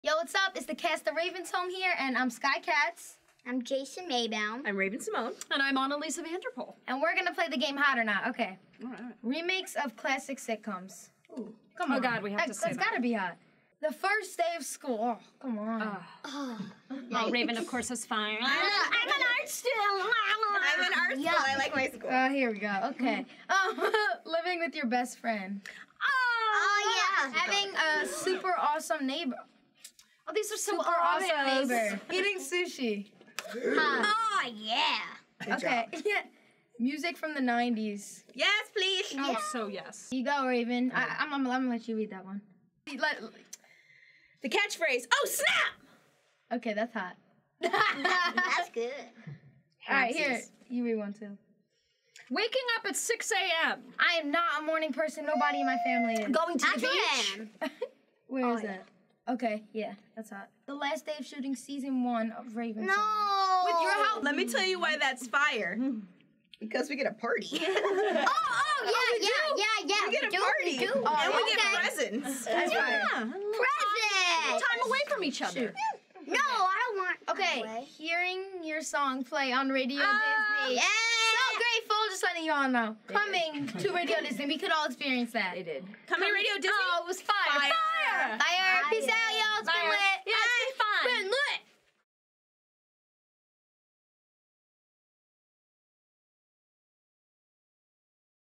Raven's Home Hot or Not | Radio Disney
Raven-Symoné, Anneliese van der Pol, Sky Katz, and Jason Maybaum from Raven's Home play Hot or Not!